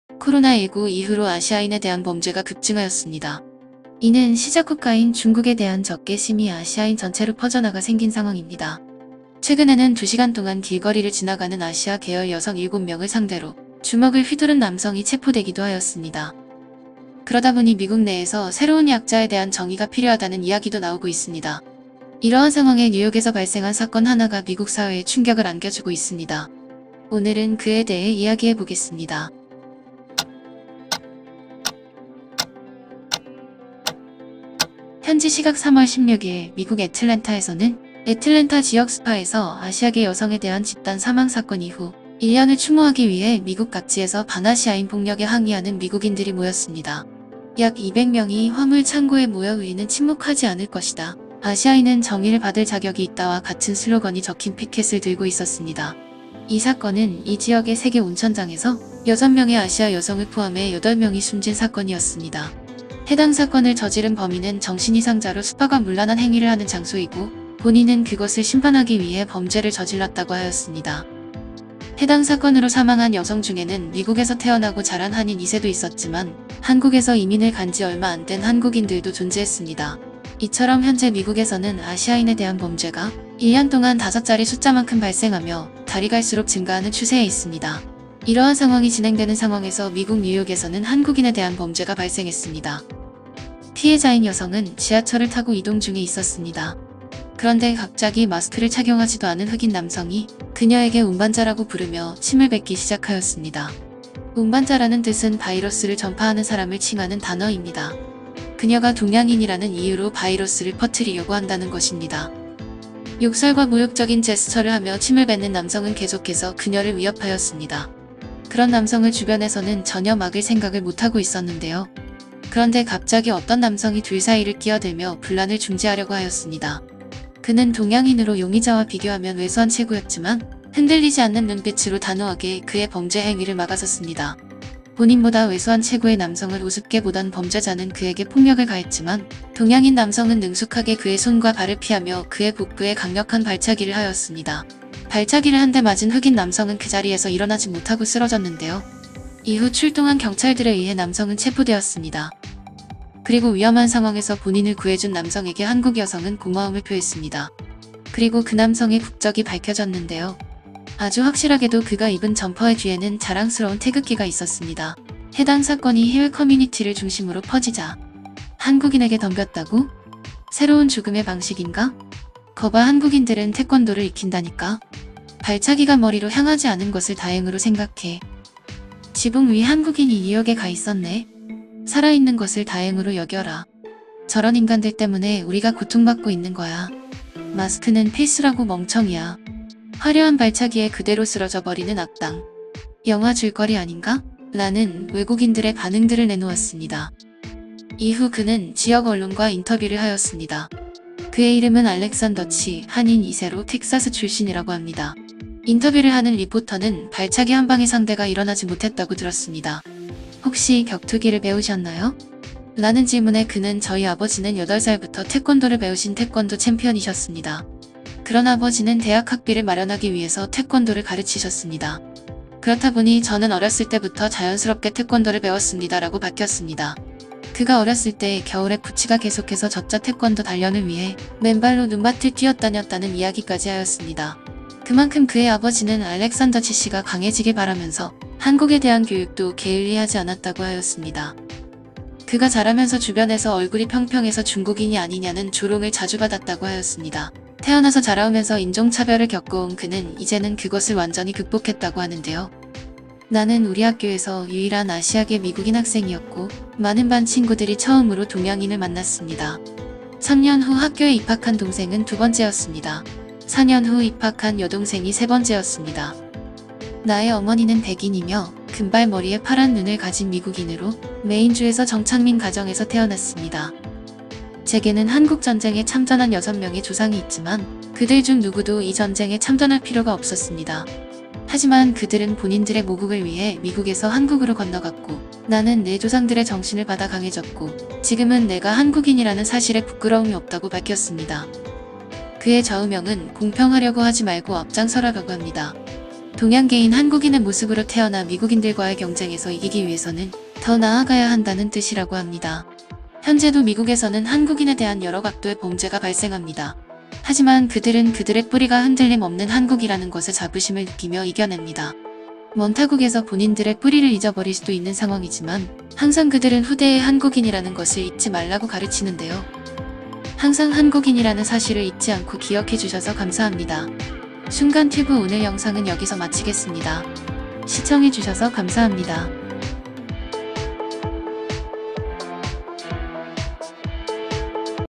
(충격영상) 지하철에서 한국여자에게 침뱉고 욕하던 흑인의 최후. 갑자기 나타난 한국남자에게 태권도 발차기로 한방!!! (해외반응)(외국반응)(실제영상)
안녕하세요~ 이 순간의 소식을 전해드리는 순간튜브입니다.

이 순간의 소식을 여러분께 전해드리겠습니다.